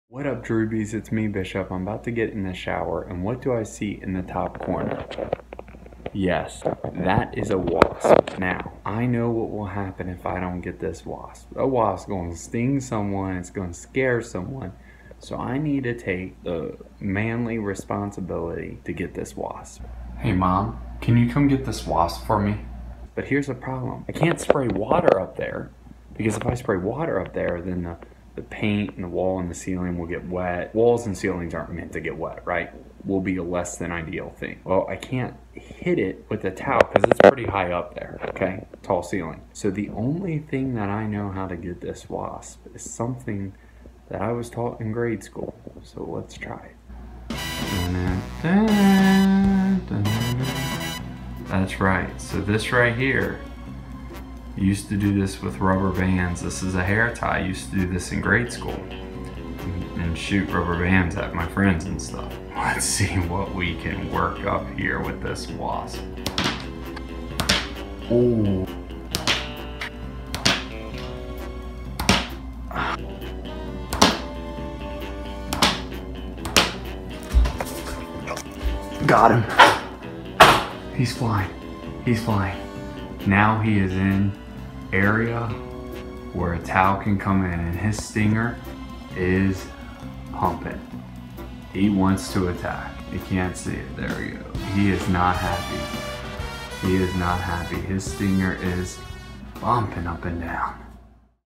A WASP INVADED MY BATHROOM!
A wasp invaded my bathroom. I didn't know what to do at first. I tried asking my mom to help, but then I realized the one thing that needed to be done.